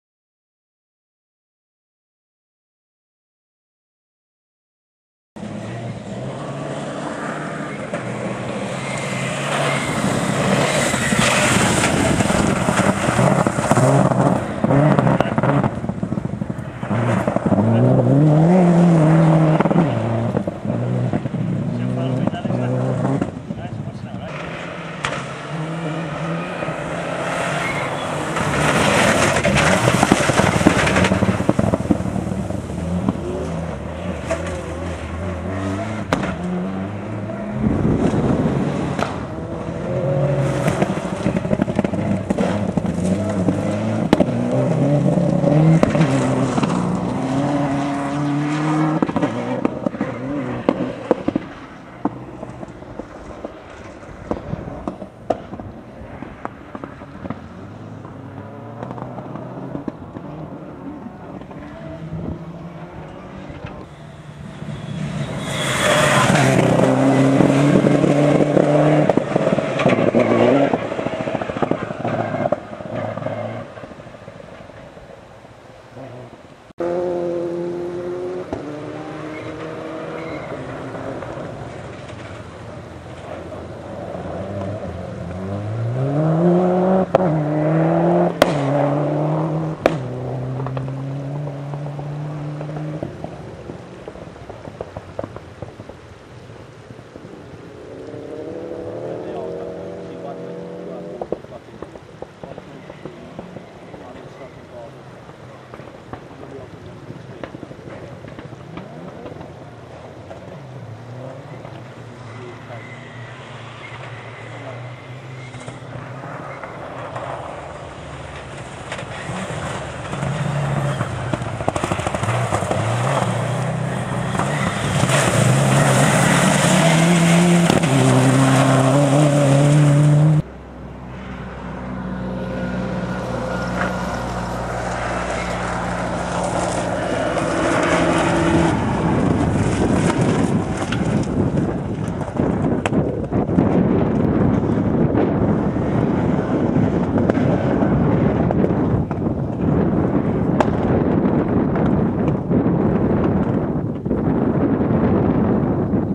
Sara Williams/Dai Roberts - Coracle Stages 2010
Footage of Sara and Dai on their way to 2nd O/a on the Coracle Stages 2010